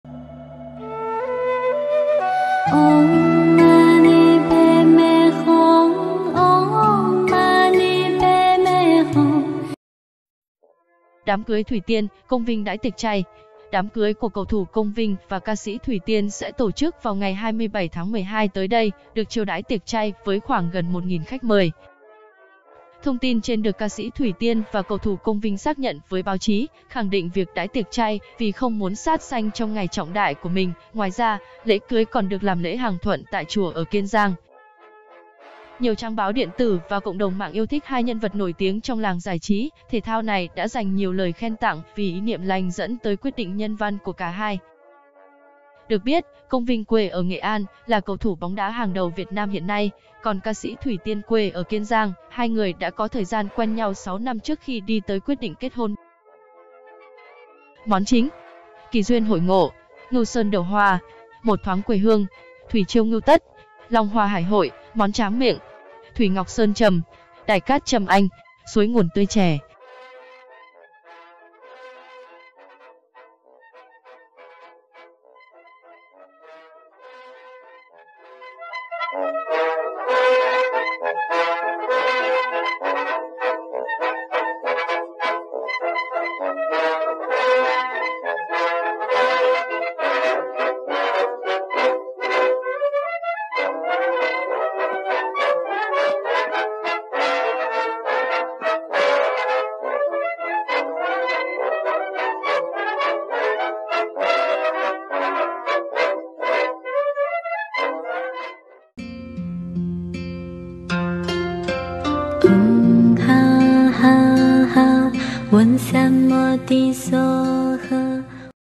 Đám cưới Thủy Tiên - Công Vinh đãi tiệc chay | Không muốn sát sinh trong ngày cưới
Đám cưới Thủy Tiên - Công Vinh đãi tiệc chay
GNO - Đám cưới của cầu thủ Công Vinh và ca sĩ Thủy Tiên sẽ tổ chức vào ngày 27-12 tới đây, được chiêu đãi tiệc chay với khoảng gần 1.000 khách mời.
Thông tin trên được ca sĩ Thủy Tiên và cầu thủ Công Vinh xác nhận với báo chí, khẳng định việc đãi tiệc chay vì "không muốn sát sanh" trong ngày trọng đại của mình. Ngoài ra, lễ cưới còn được làm lễ hằng thuận tại chùa ở Kiên Giang.
Nhiều trang báo điện tử và cộng đồng mạng yêu thích hai nhân vật nổi tiếng trong làng giải trí - thể thao này đã dành nhiều lời khen tặng vì ý niệm lành dẫn tới quyết định nhân văn của cả hai.
Được biết, Công Vinh quê ở Nghệ An, là cầu thủ bóng đá hàng đầu VN hiện nay, còn ca sĩ Thủy Tiên quê ở Kiên Giang; hai người đã có thời gian quen nhau 6 năm trước khi đi tới quyết định kết hôn.

Em bé ăn chay trường từ trong bụng mẹ theo Thực Dưỡng, vợ chồng ca sĩ Nguyễn Đức Tùng